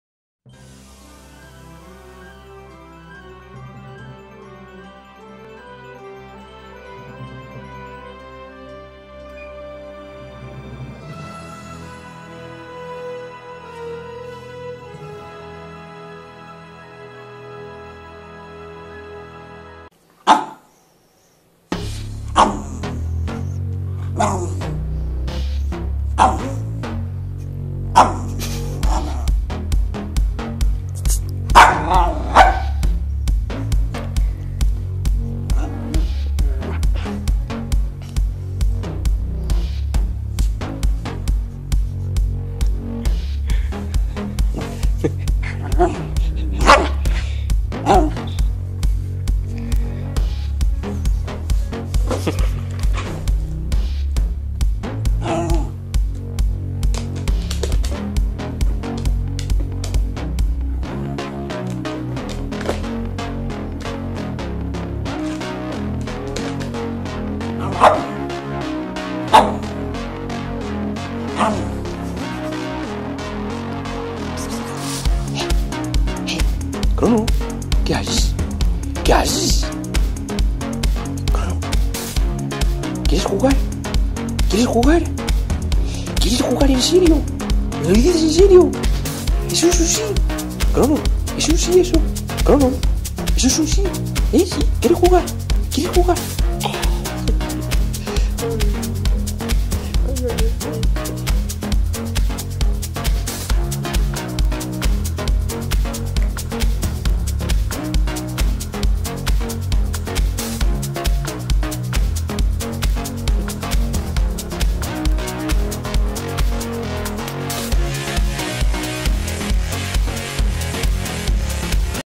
My dog Yorkshire ​​Crono wants to play / Crono mi perro quiere jugar
Estaba sentado con el ordenador cuando derrepente Crono, mi perro, quería jugar :-D